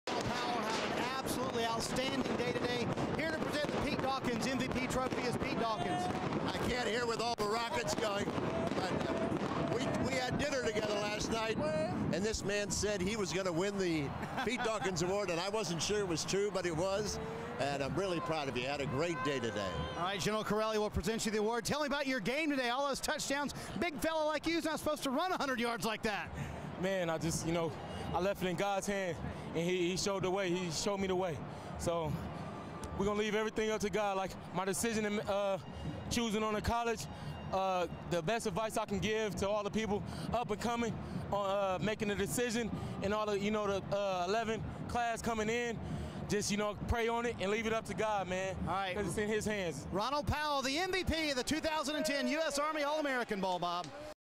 2010 Army Bowl | Ronald Powell MVP
With a stat line that included two trips to the endzone (1 receiving TD, 1 extra point return), a sack and a forced fumble, Powell is awarded the Pete Dawkins trophy as the game's MVP.